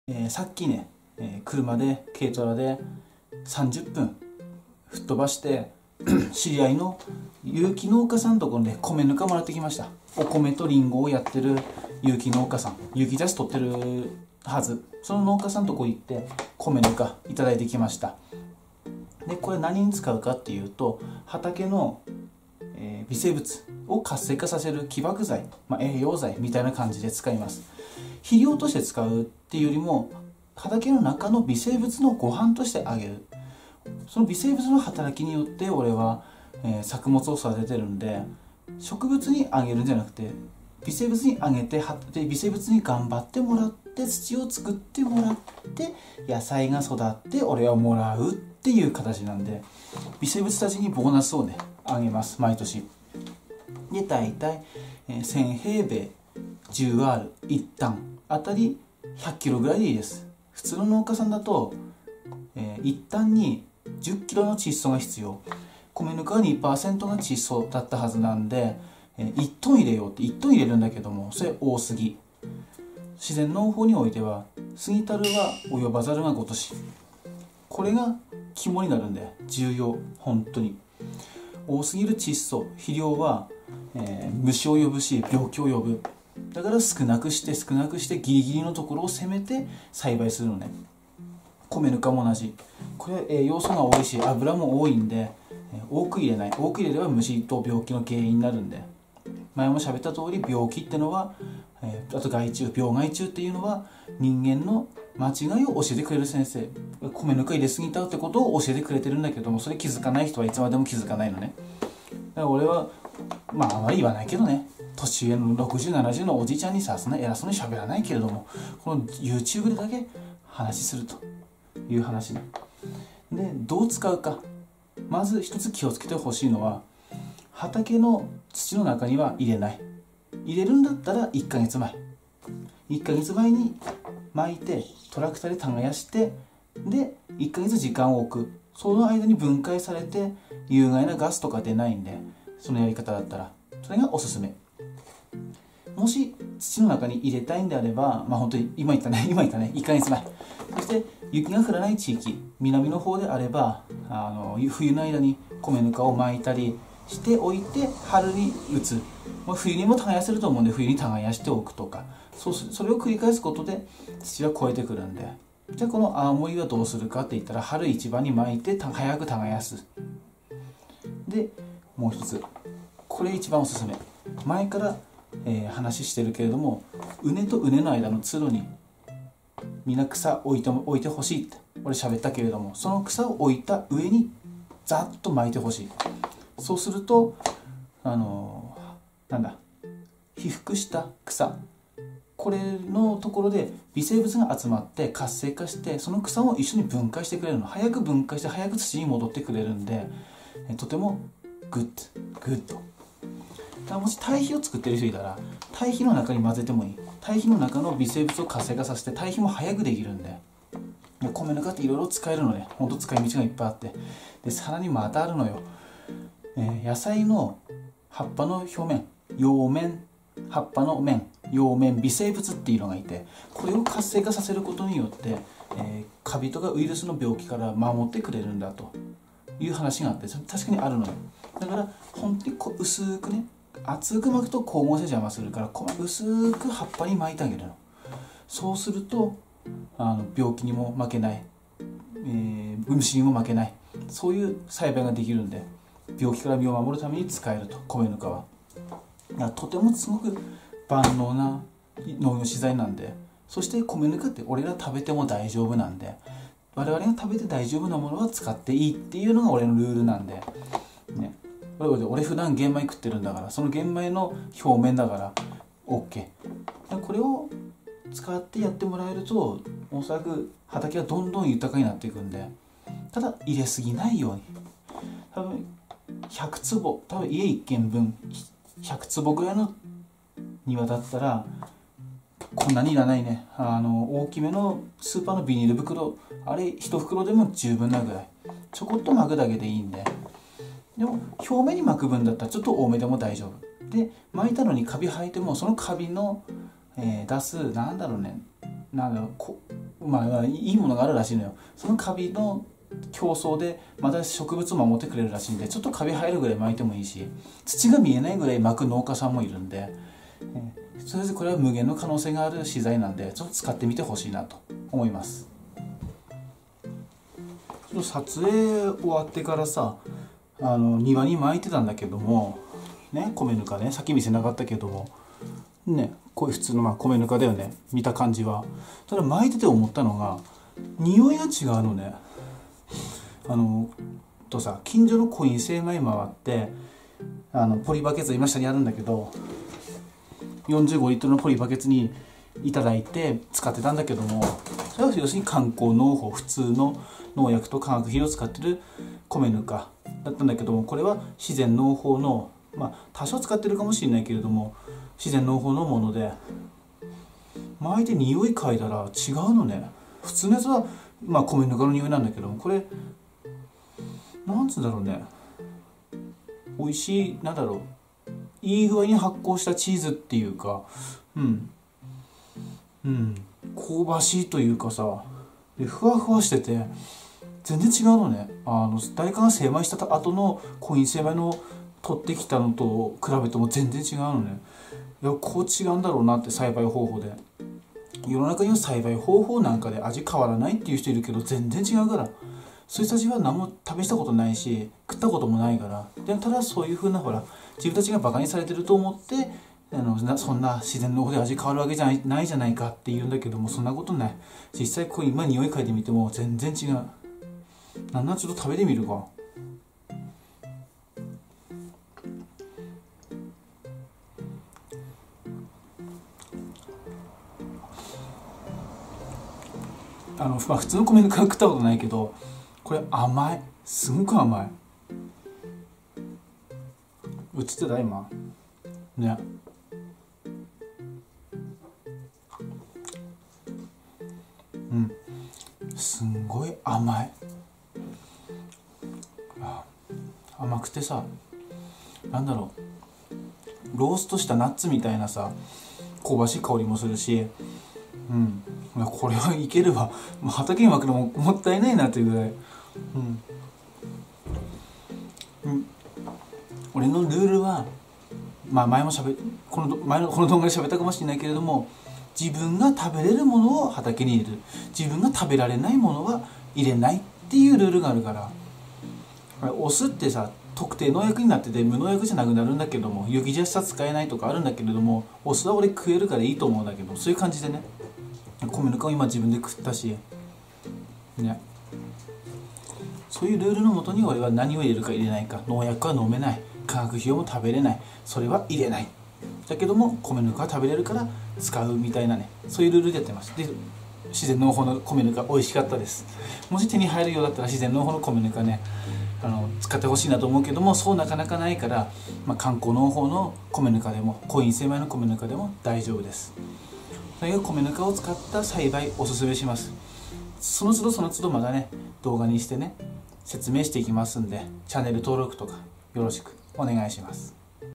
自然農法の米ぬか入手したので活用法を紹介します
知り合いの農家さんから自然農法の米ぬか300㎏いただきました。
畑に使うんですが使い方にはいくつか注意点があるのでご紹介します。

量は少なめに。
どんな資材でもそうですが多すぎると病害虫を呼びます。「過ぎたるは猶及ばざるが如し」と思い出してください。

土には混ぜない。混ぜるなら植え付け一か月前。
生の有機物は土に混ぜるとガスを発生させて根を痛める恐れがあるので、注意してください。植え付けまで一か月の猶予があれば、ほぼ分解されるので大丈夫です。夏場なら3週間でいいかも。
基本的には通路に敷いた刈草や野菜くずの上に散布してください。
好気的条件で使いましょう。

葉面散布。
葉っぱの表面に住む微生物を活性化させる作用もあるので、うすーく葉っぱにかけてあげてください。かけたらふるい落とす感じでもいいです。
葉面微生物を活性化させることで、害なす微生物から守ってあげます。

畑には自分が食べても大丈夫なものを入れるようにしましょう。
米ぬか美味しかったです。